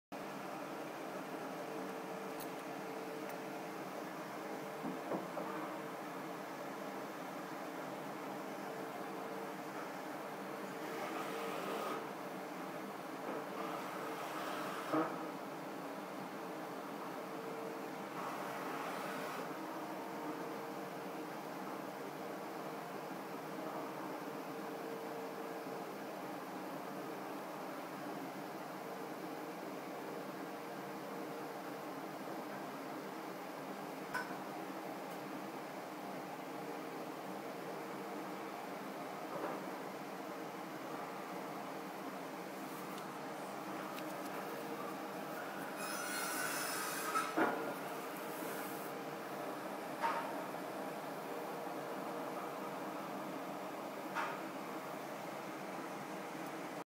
Man In A Bubble
Trapped inside as floor underlayment is replaced outside my door.